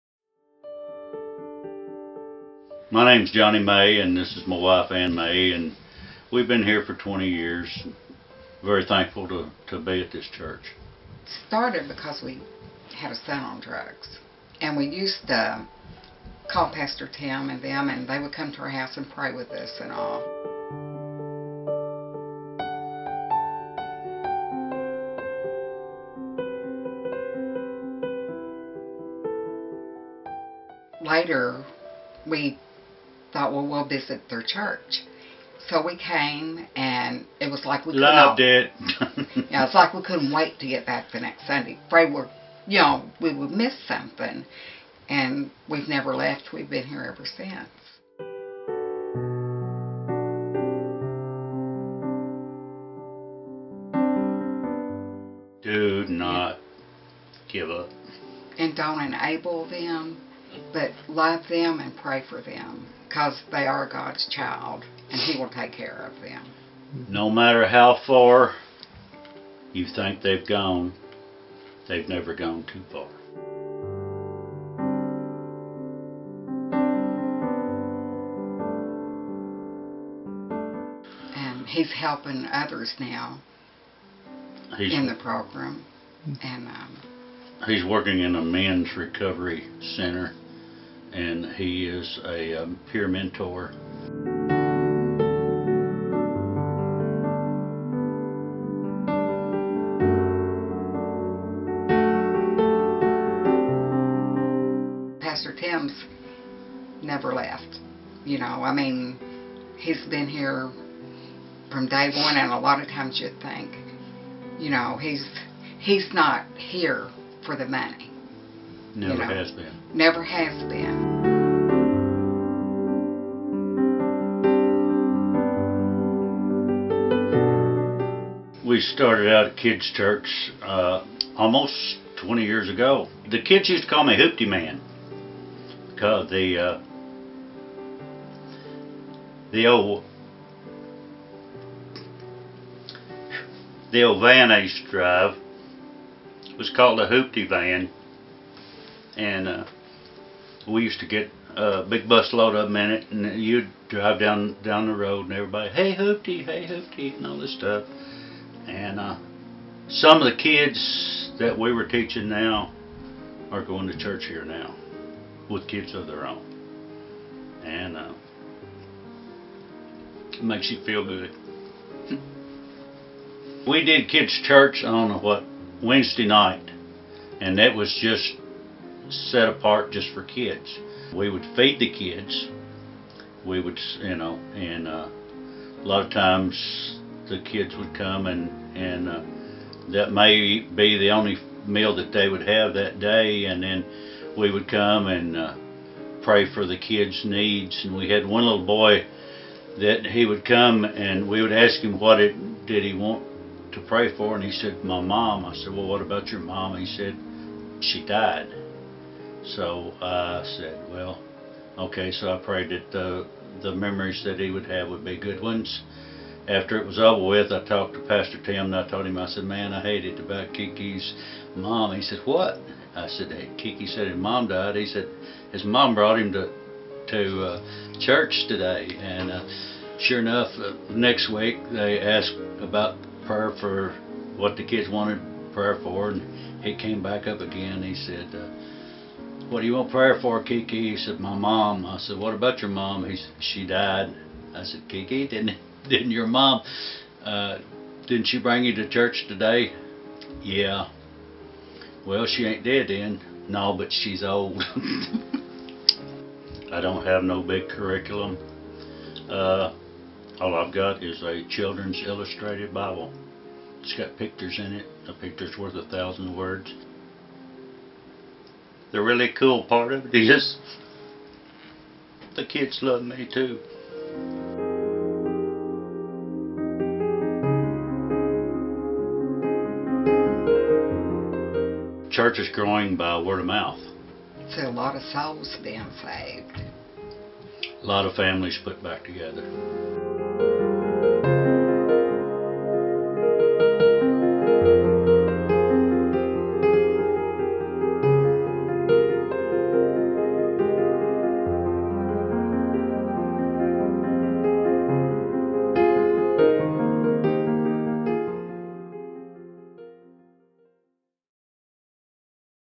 Johnny and Ann May Testimony
*MUST SHARE* - In the midst of dealing with their own child with addiction, Johnny & Ann May reach out to the children in their community. Couples heartfelt testimony will encourage and motivate you.

God is moving in the lives of our people at New Covenant. Here is one couple's story.

New Covenant Christian Center is located at 110 E Main St, Providence (Webster County), Kentucky.

Pastors Tim and Barbara Rigdon.